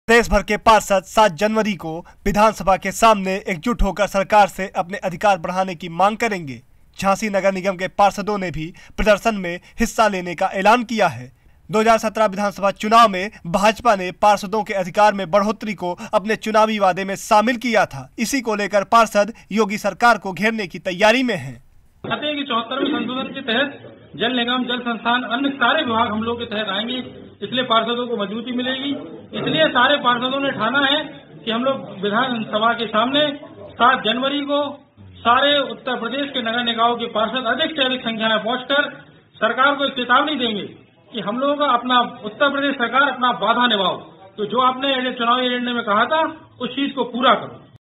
7 को राजधानी में जुटेंगे प्रदेश भर के पार्षद, अधिकार बढ़ाने की करेंगे मांग
झांसी : प्रदेश भर के पार्षद सात जनवरी को विधानसभा के सामने एकजुट होकर सरकार से अपने अधिकार बढ़ाने की मांग करेंगे. झांसी नगर निगम के पार्षदों ने भी प्रदर्शन में हिस्सा लेने का ऐलान किया है. विधानसभा चुनाव में भाजपा ने पार्षदों के अधिकार में बढोत्तरी को अपने चुनावी वादे में शामिल किया था. इसी को लेकर पार्षद 74वां संविधान संशोधन कर अधिकार बढ़ाने के साथ ही मानदेय और भत्तों जैसी मांगों को लेकर आवाज बुलंद करने की तैयारी में हैं.